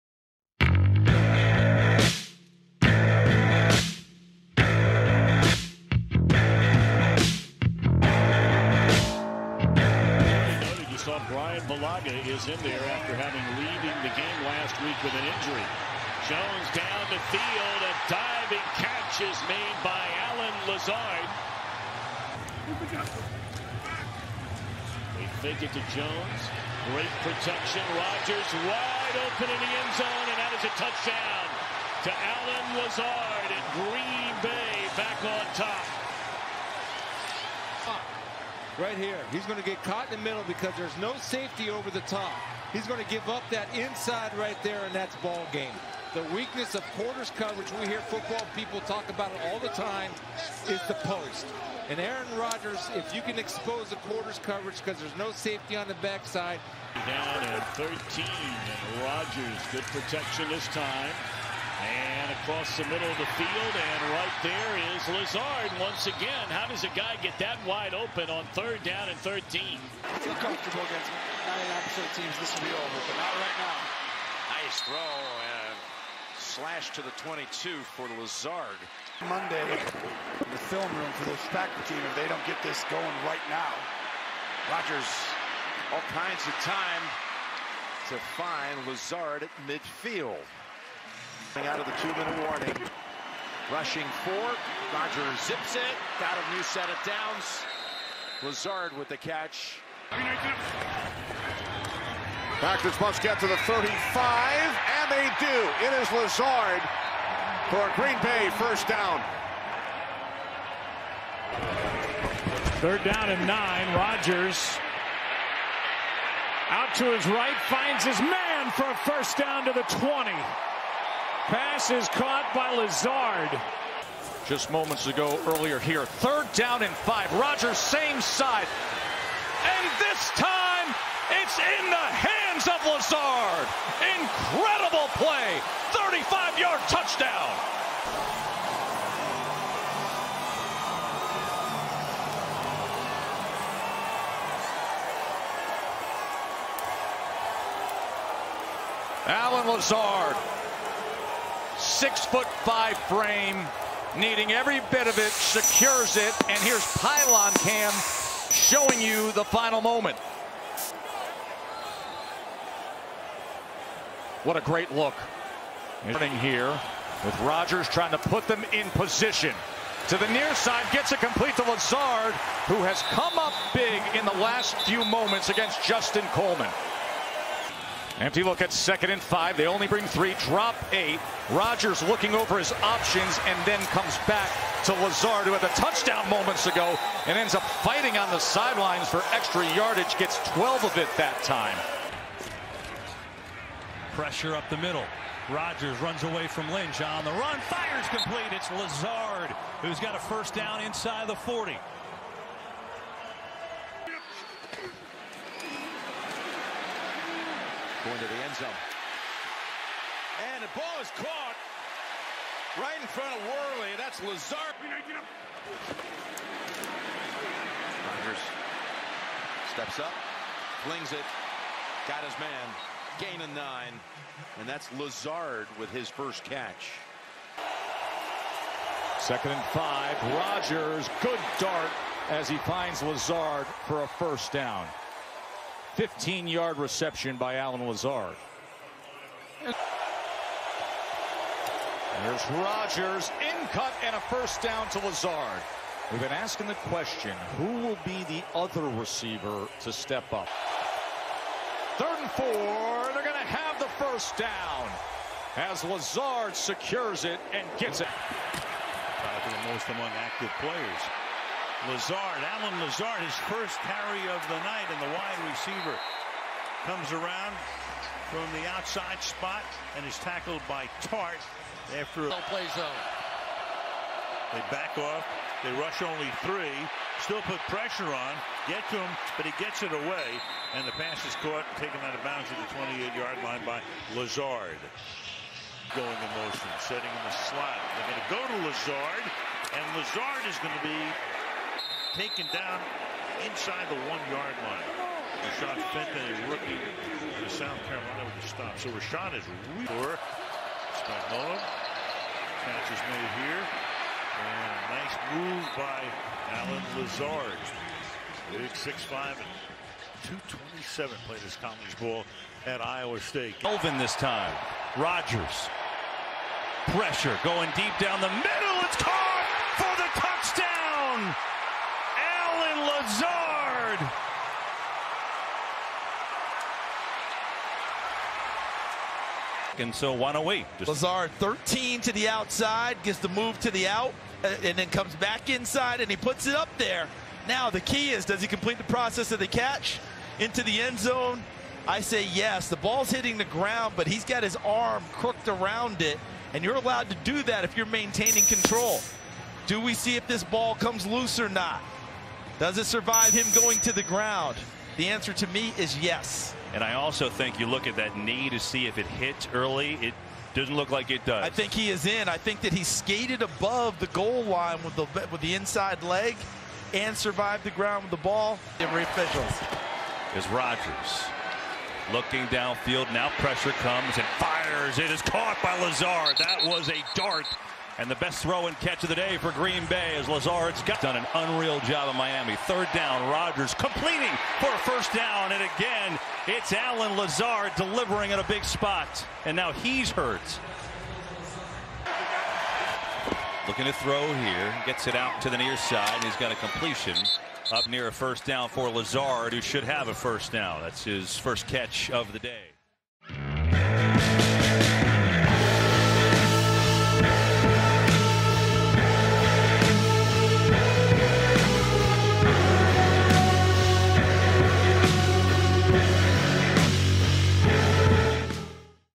Allen Lazard Highlights ᴴᴰ 2019 Season | Green Bay Packers Highlights | Allen Lazard Fantasy
Green Bay Packers MERCH - ON SALE NOW

Allen Jamel Lazard (born December 11, 1995) is an American football wide receiver for the Green Bay Packers of the National Football League (NFL). He played college football at Iowa State, and was originally signed by the Jacksonville Jaguars as an undrafted free agent in 2018.

Lazard was a top-rated recruit coming out of high school, where he was named a 2013 USA Today High School All-American. Lazard has been selected to the First-team All-Big 12 in both 2016[3] and 2017 and is Iowa State's career leader in both receptions and receiving yards.

2019 Season

Lazard recorded his first professional touchdown, a 35-yard reception from Aaron Rodgers, during a Week 6 win over the Detroit Lions on October 14, 2019. He had a breakout performance on December 1, 2019 in a 31–13 week-13 victory over the New York Giants, posting three catches for 103 yards and a touchdown. Overall, Lazard finished the 2019 season with 35 receptions for 477 receiving yards and three receiving touchdowns, while also adding one rushing attempt for 21 yards.